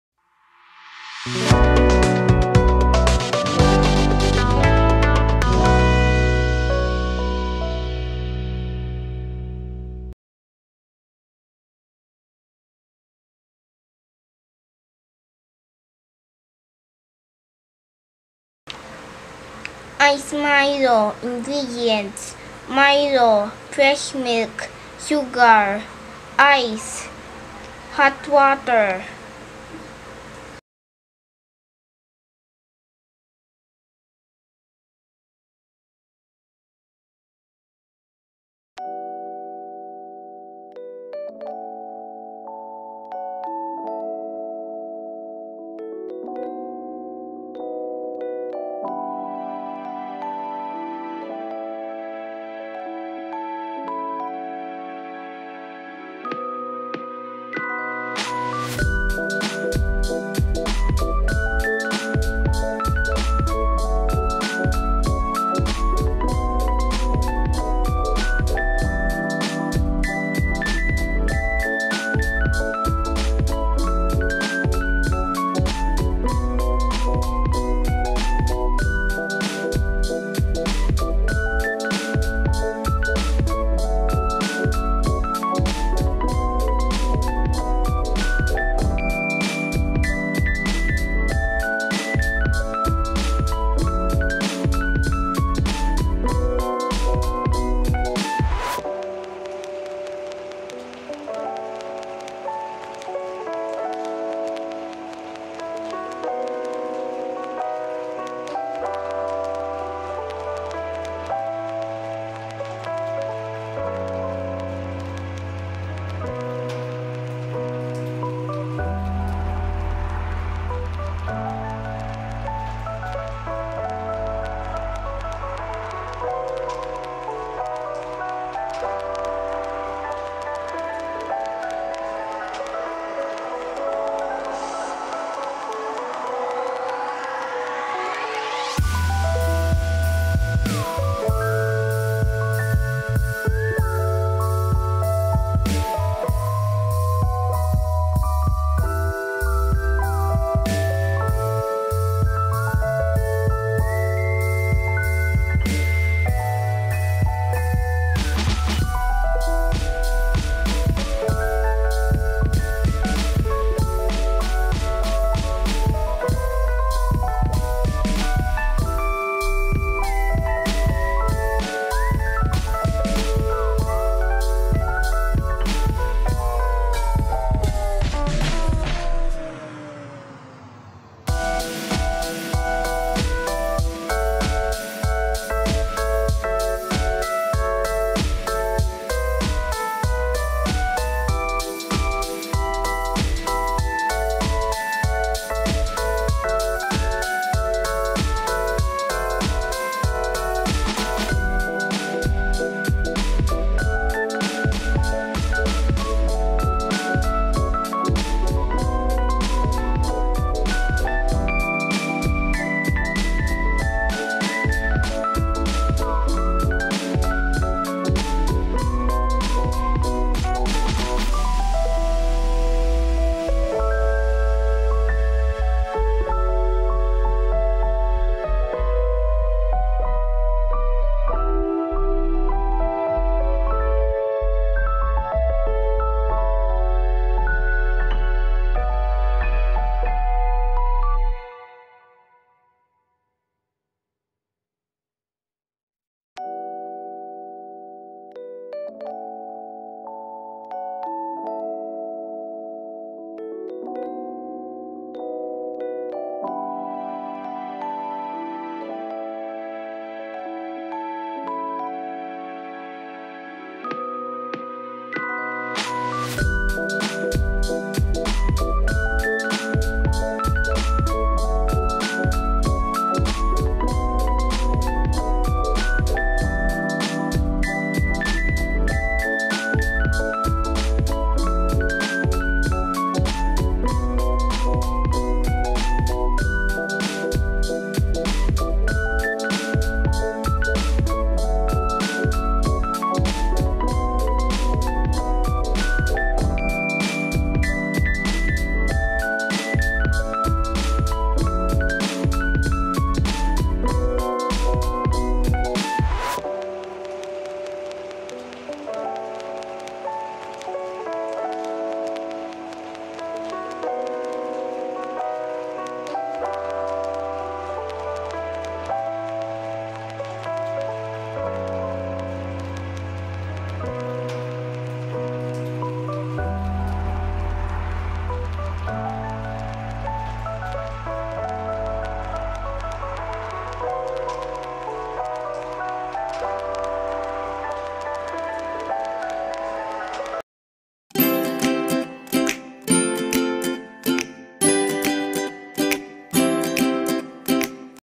ICE MILO | MILO ICE IN MILK # COLD DRINK
ICE MILO By: Mycah & Myra

Ice Milo is simply a cold Milo drink with water or milk, ice cubes and sugar, topped with more Milo powder. For a more chocolatey taste, you can add cocoa powder.
ingredients. This is the basic Milo recipe. Make it according to package directions, or customize it to fit your personal taste. For this recipe you will need:
• 3 tablespoons Milo powder
• 2 tsp Sugar
• 2 tsp Hot water
• 2 cups lukewarm fresh milk
• Ice (cube ice)